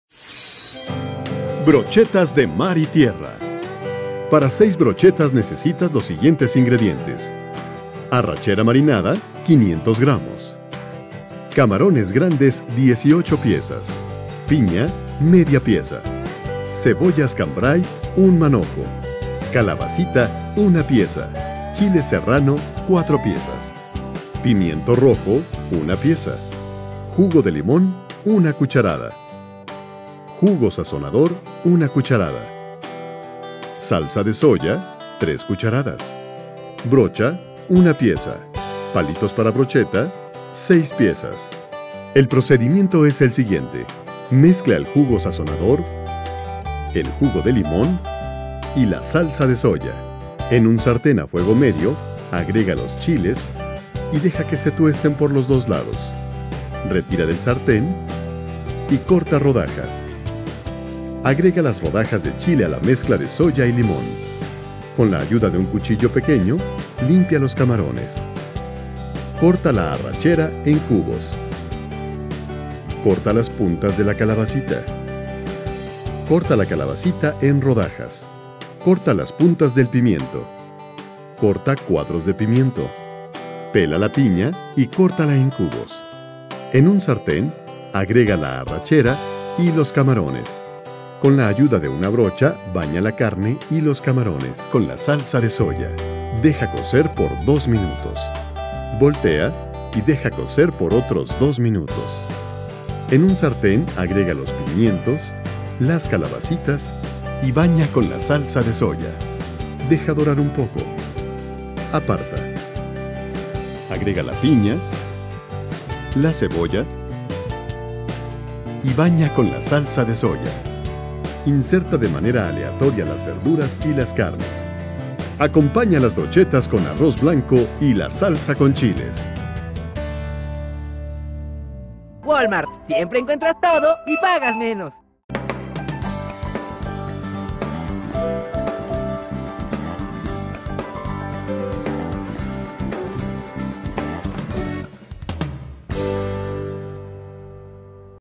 ¿Cómo preparar Brochetas de Mar y Tierra? - Cocina Fresca
Para 6 brochetas necesitas :
Arrachera Marinada 500g
Camarones Grandes 18pzas
Piña 1/2 pza
Pimiento Rojo 1 pza
Calabacita 1 pza
Cebollas Cambray 1 manojo
Chile serrano 2 pzas
Jugo Sazonador 1 cda
Salsa de Soya 3 cdas
Jugo de limón 1 cda
Arroz blanco para acompañar
Brocha 1 pza
Palitos para brocheta 6 pzas
Procedimiento:
Mezcla el jugo sazonador, el jugo de limón y la salsa de soya
En un sartén a fuego medio agrega los chiles y deja que se tuesten por los dos lados
Retira del sartén y corta rodajas
Agrega las rodajas de chile a la mezcla de soya y limón
Con la ayuda de un cuchillo pequeño, limpia los camarones
Corta la arrachera en cubos
Corta las puntas de la calabacita
Corta rodajas
Corta las puntas del pimiento
Corta cuadros de pimiento
Pela la pina y corta cubos de piña
En un sartén, agrega los camarones y la arrachera
Con la ayuda de una brocha, baña la carne y los camarones
Deja cocer 2 minutos
Voltea y deja otros 2 minutos
Aparta
En un sartén agrega los pimientos, las calabacitas y la piña
Baña con la mezcla de soya
Deja dorar un poco
Aparta
Inserta de manera aleatoria las verduras y las carnes

Todo lo necesario para hacer esta receta lo puedes encontrar en Walmart